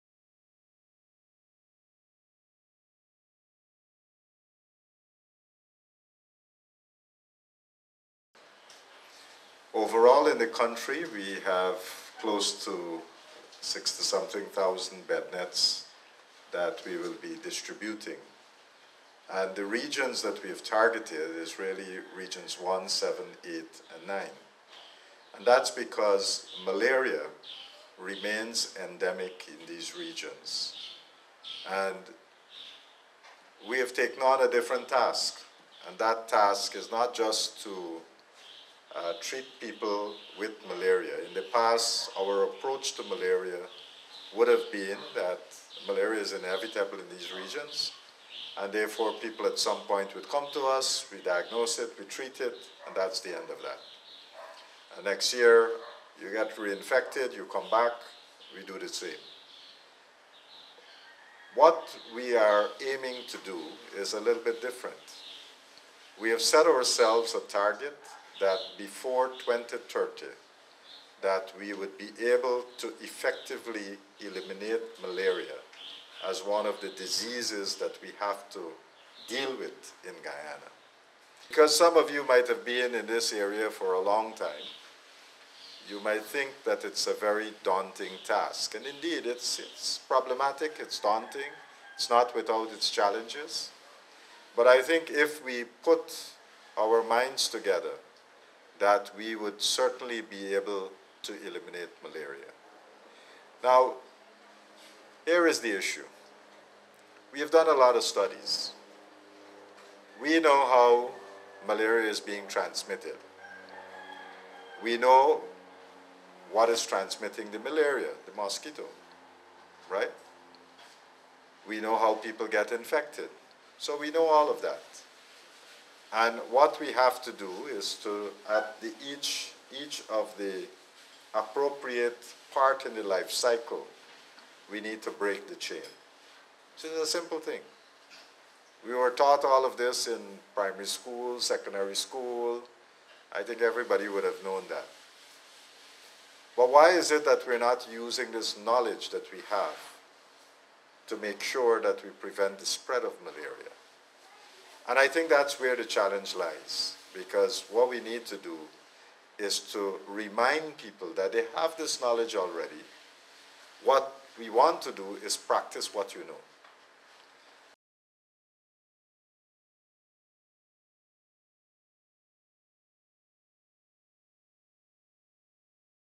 HEALTH MINISTRY HOPES TO RID COUNTRY OF MALARIA BY 2030
Malaria, a potentially life-threatening but curable disease spread to humans by mosquitoes, substantially affects people living in Guyana’s Indigenous regions. Cognisant of the burden of this disease, the Ministry of Health is distributing treated mosquito nets in at-risk communities so that people can be protected from the mosquitoes. As he helped to distribute these nets in Region Eight, Health Minister Dr. Frank Anthony said the local health authorities have an ambitious goal of eliminating malaria from Guyana by 2030.